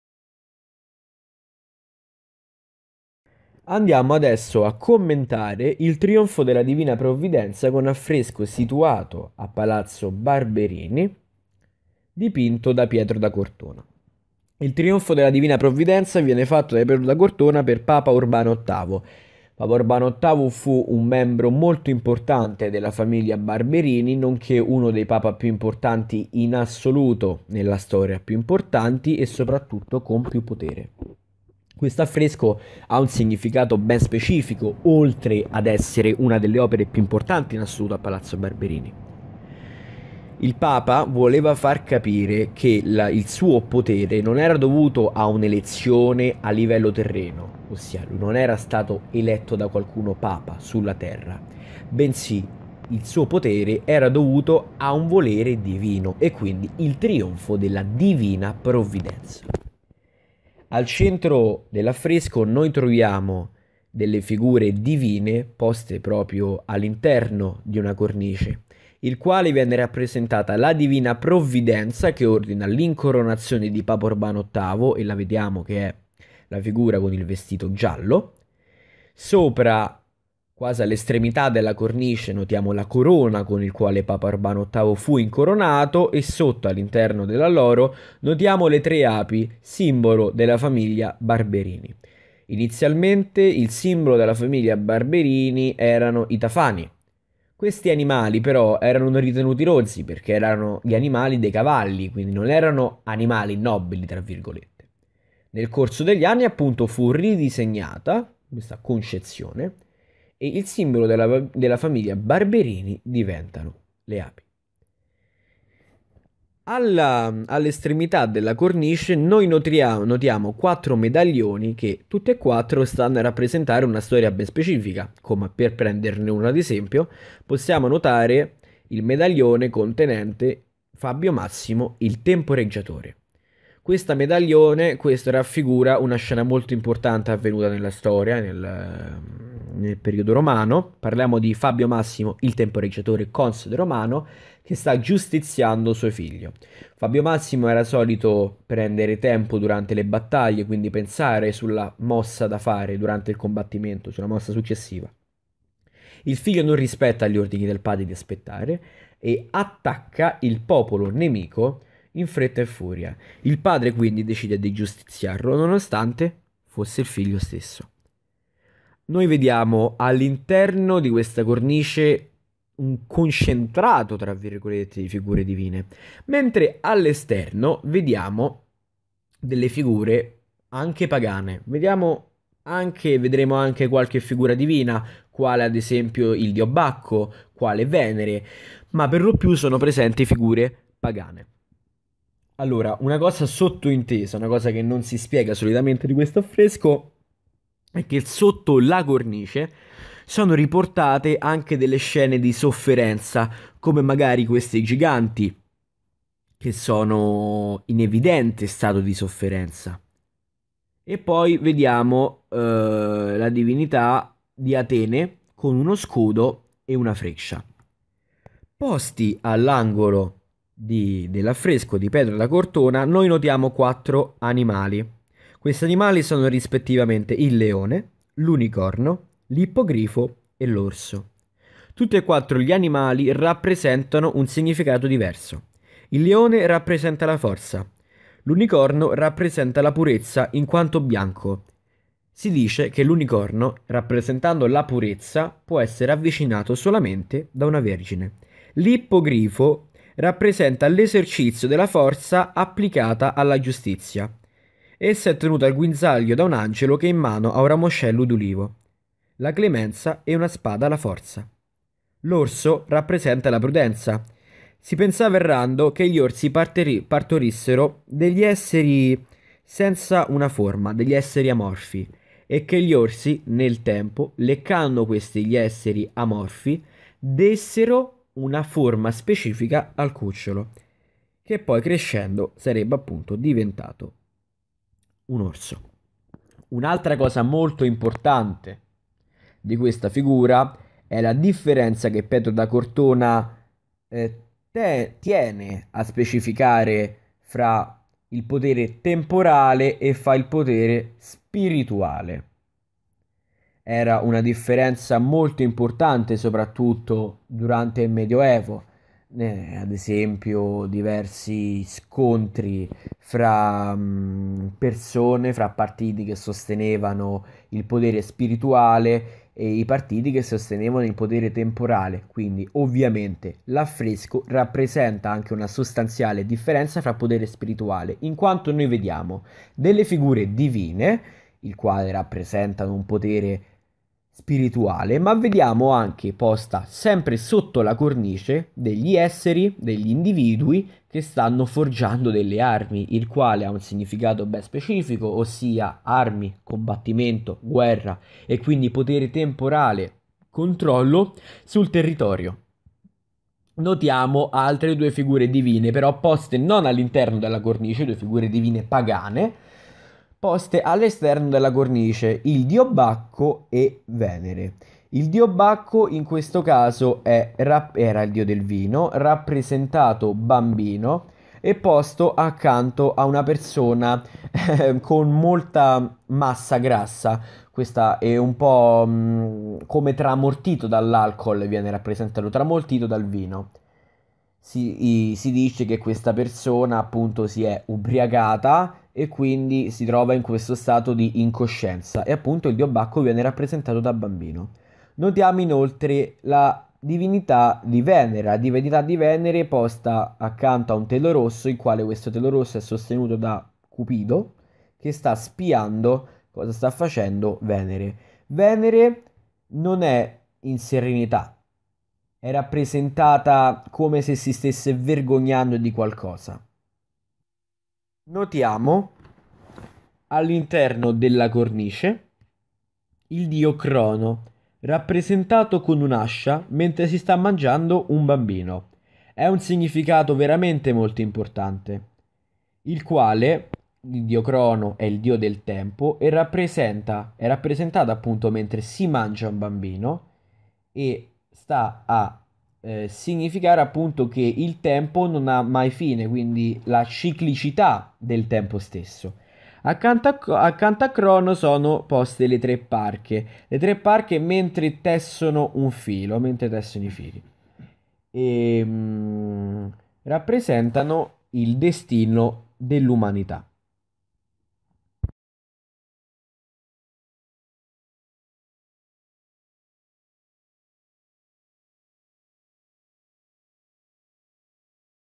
Pietro da Cortona: Il trionfo della Divina Provvidenza.
Progetto ASL: Il trionfo della Divina Provvidenza, commento dell'opera.
Storia dell'arte.
Hanno preso parte alla realizzazione del video: Binotti Lorenzo(III D SAP) e Ceruzzi Luciano(III D SAP)